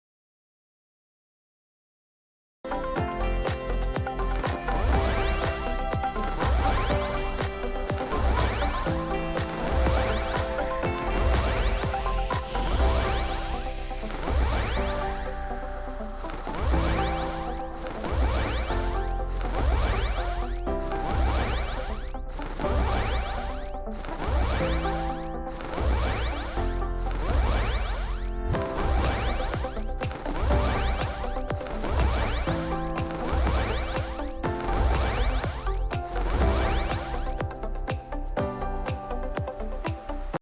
Fruiting UlforceVeedramon X
Lets see what size I can get for UVX